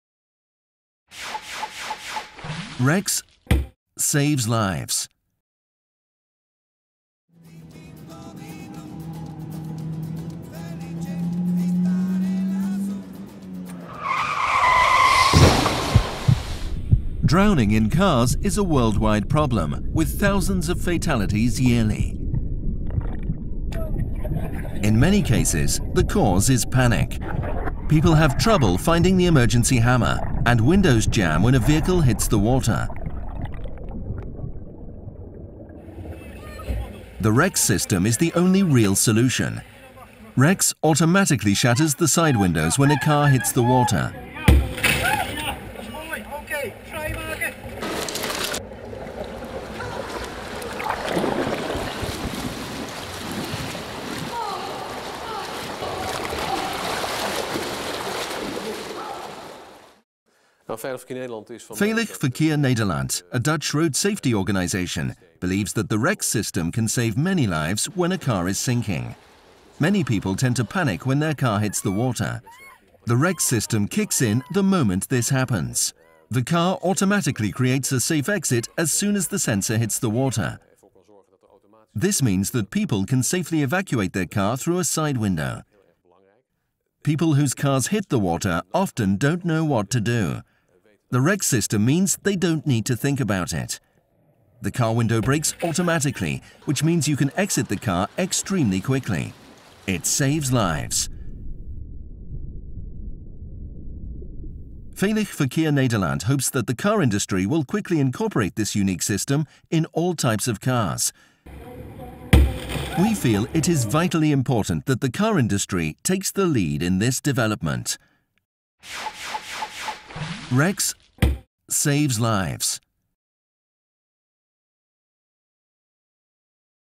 SAS - Rescue Escape Guidance System (REGS)
REGS consists of water detection sensors and several activators for crashing the windows. The sensors are situated inside the car. Because of the type of sensors and its positioning, it can be guaranteed that the system will only be activated when the inner space gets filled to a certain level. Integrated is technology to guarantee the functioning of the system in case of upside down landing.

The REGS team is open for a license based corporation with both the OEM and the aftermarket car service companies.

For more information regarding the REGS Safety System, please refer to the following